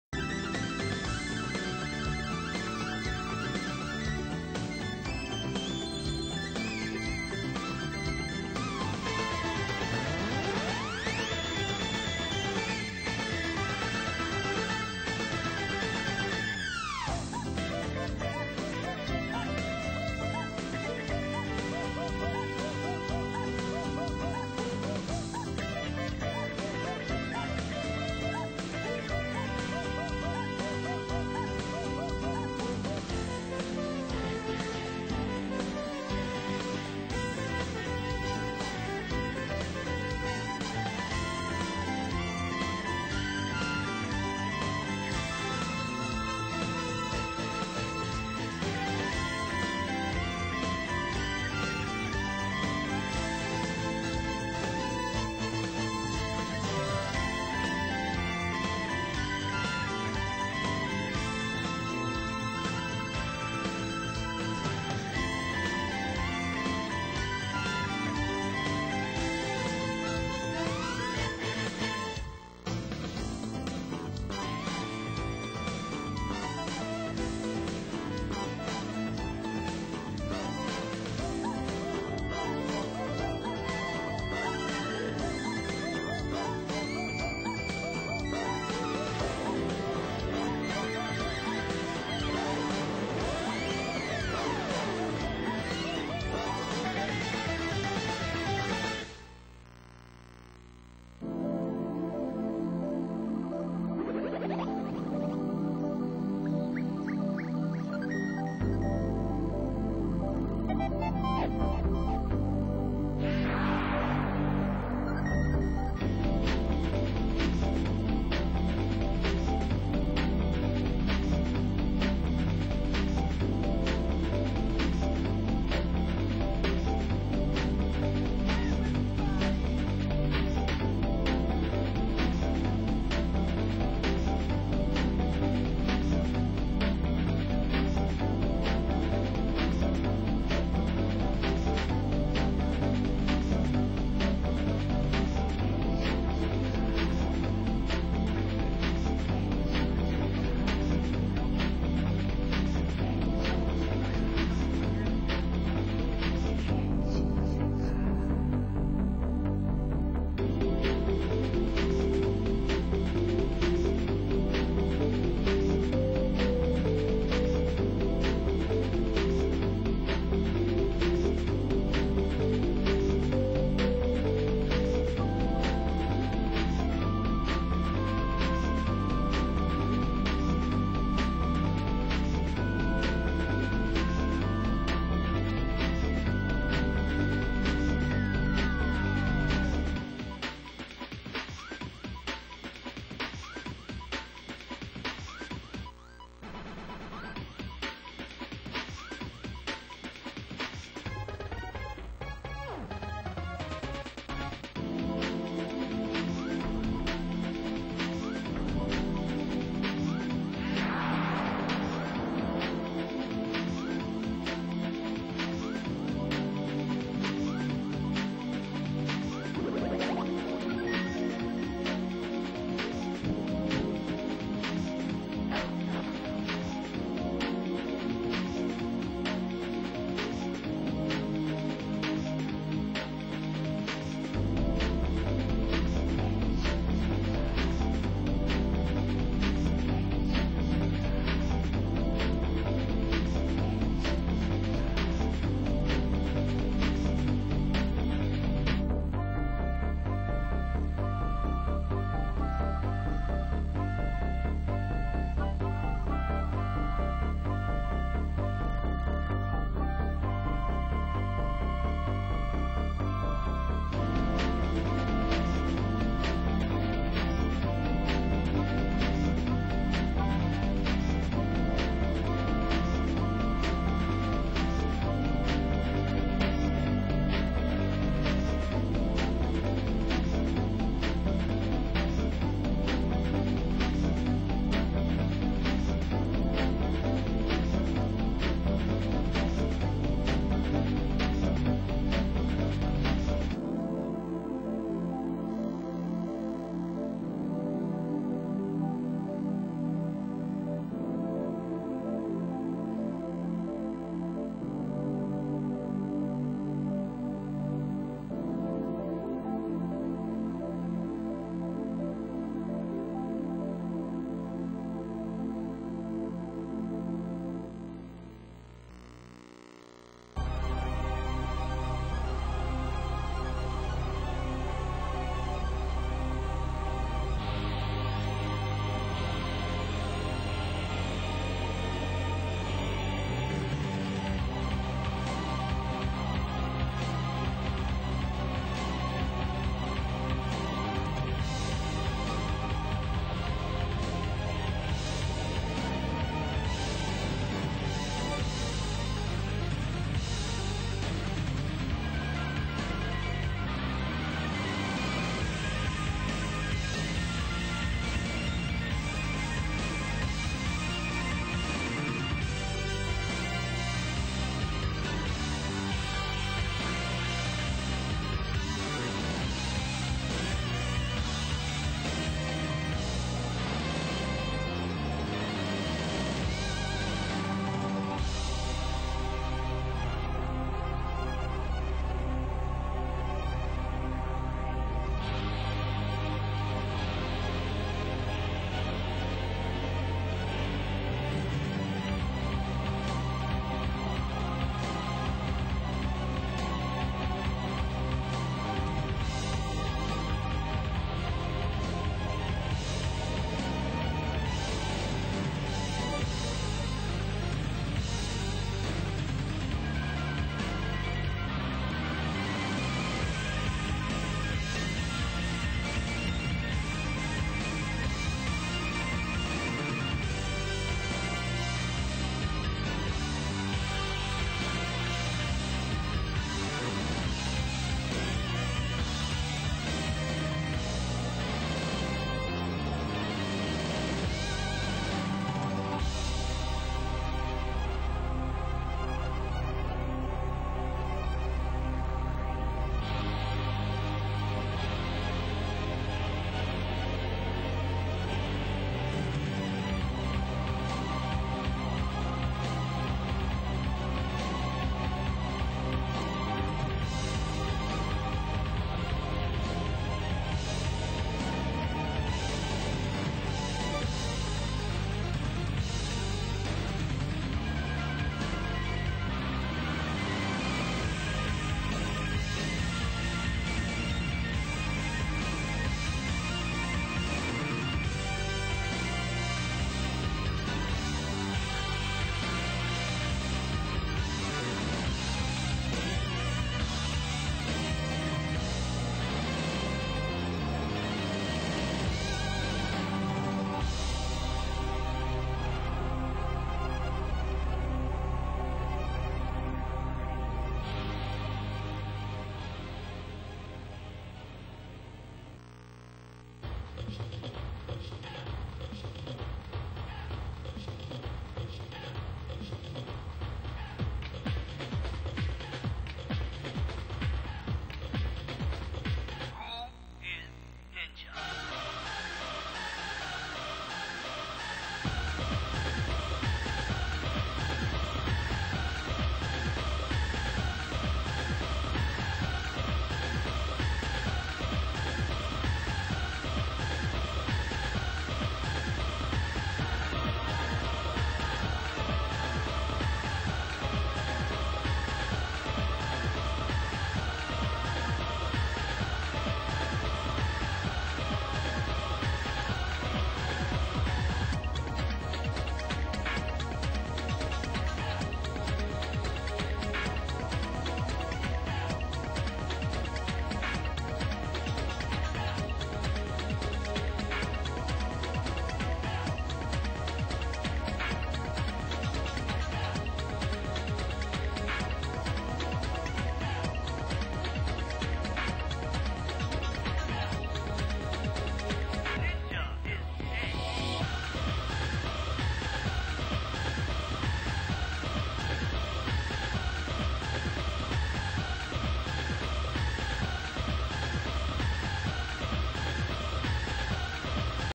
1994 Winter CES 4/6
The legend continues: the full 1994 Winter CES (games portion) video I put together all those years ago. The show was at the Las Vegas Convention Center and was the second CES/E3 I attended. Debut of the ill-fated Amiga CD32.

No tripod, no steady cam, no direct feed. Just hand-held footage edited together on a couple of VCRs with a soundtrack featuring original and arranged versions of contemporary game soundtracks. Can you name them all?